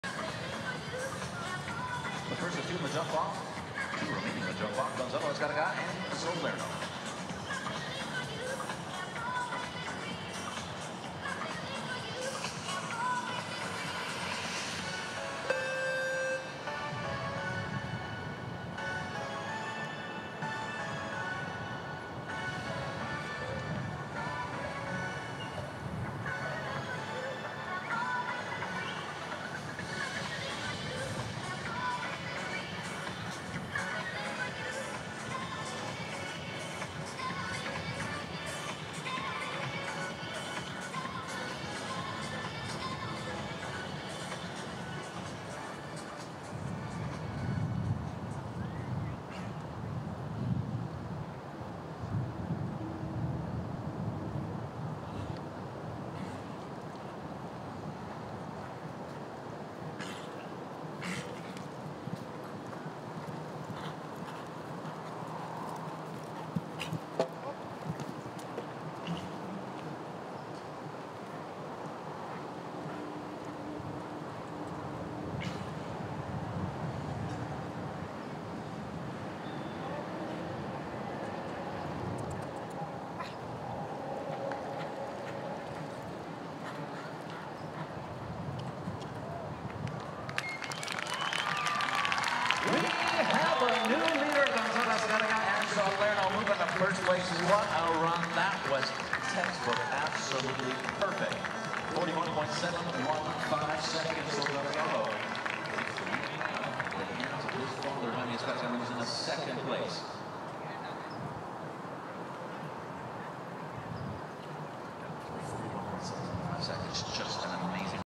Gonzalo Azcarraga and Zalerno, winners of the $25,000 Markel Insurance Grand Prix
Blenheim EquiSports - 2018 Showpark August Festival, Del Mar Horse Park, courtesy of MyHorse.Video